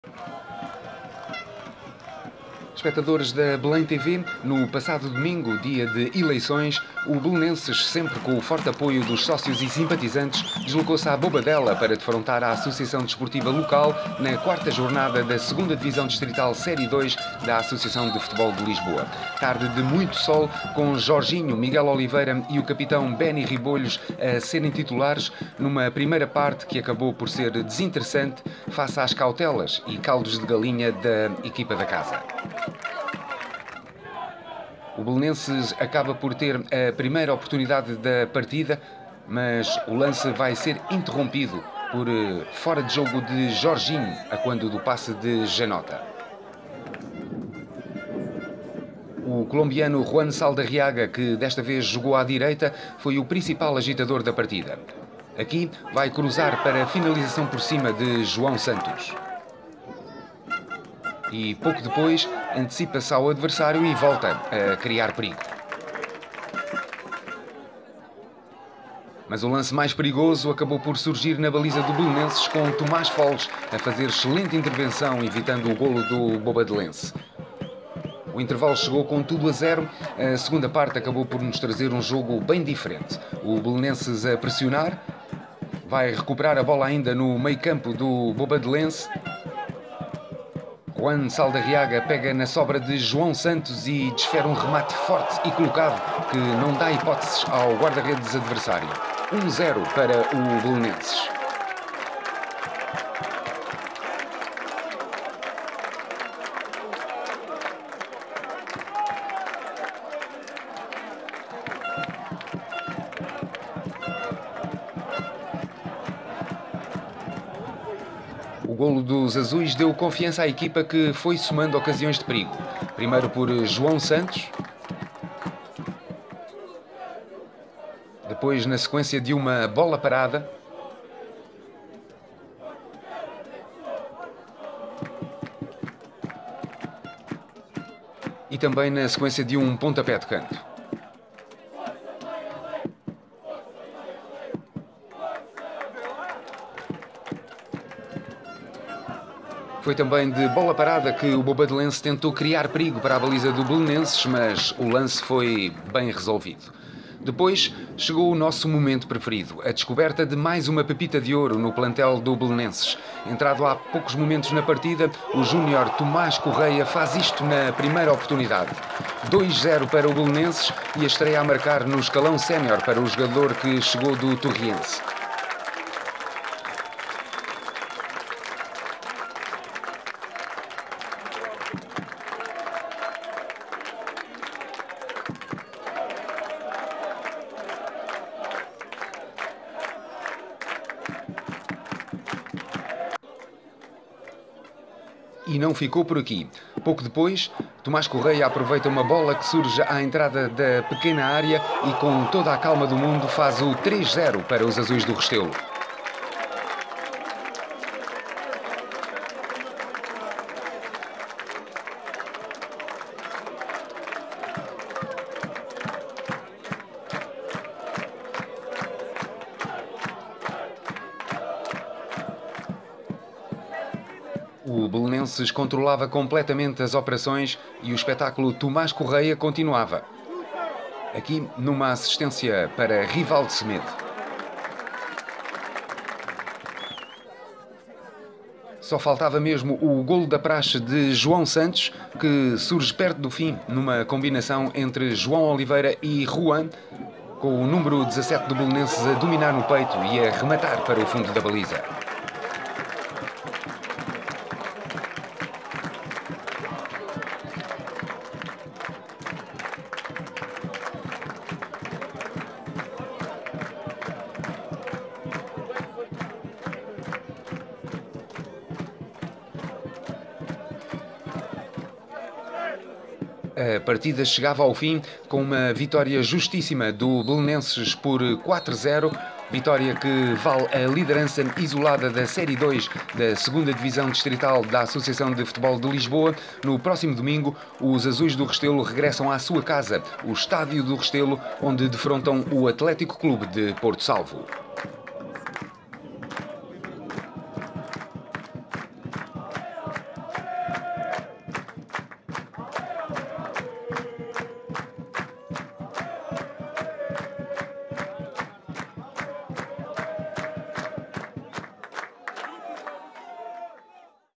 Bobadelense - Belenenses | O resumo na Belém TV
O Belenenses somou a quarta vitória em quatro jornadas mantendo a liderança isolada do campeonato ao vencer o Bobadelense por 0-4, no passado domingo, com golos de Juan, Tomás Correia (2) e João Santos!

Ocasião para conferir o resumo do jogo em mais um momento Belém TV.